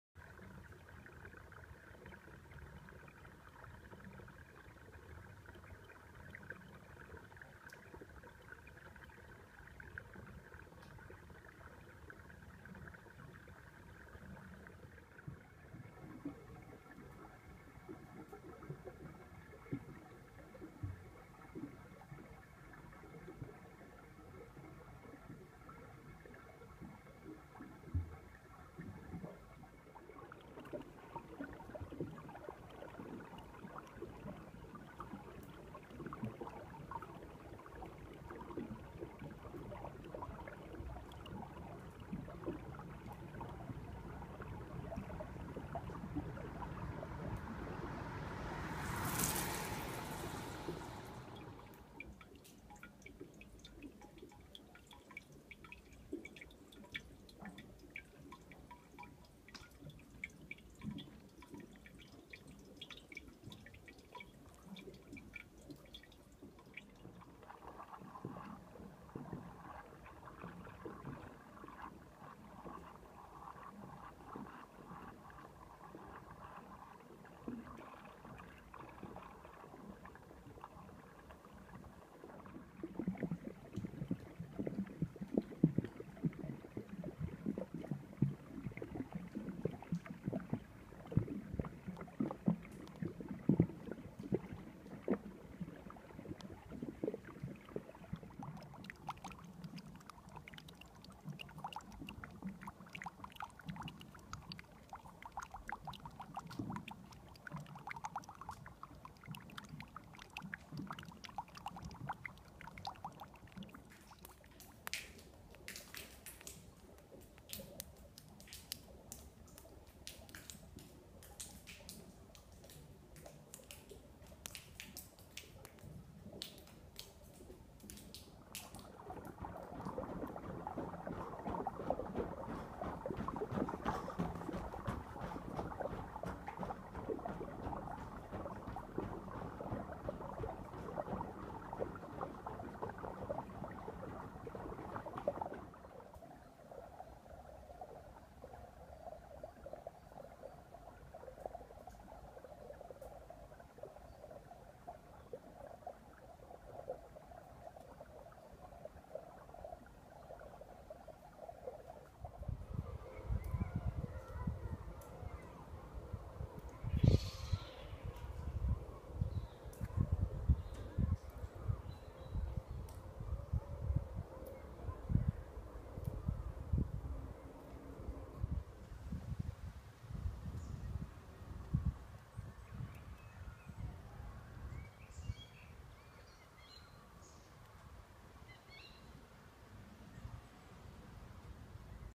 Listening to Devonshire Tunnel
The gurgles and burbles in the drains in the Devonshire Tunnel, part of the Two Tunnels Greenway in Bath.

These sounds are as you hear them in the tunnel. I'd never noticed them before as I'm usually running or cycling through.

Recorded on iPhone 5s